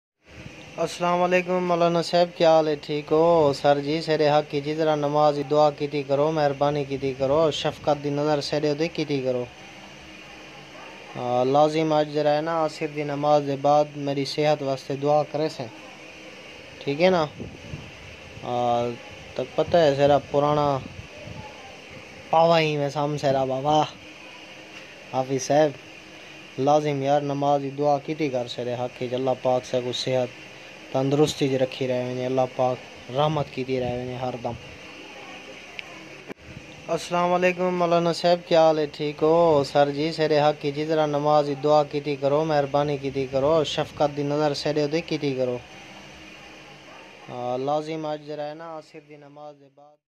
Sakhi sarwar ka shehri roshan javed ki apni zindage ki akhri recording || News live
Roshan javed Shaikh kanri ala ki zindage ki akhri guftgu. Apni sehat k liye dost se dua karny ka keh rahy hain .jis K bad onka intaqal hogaya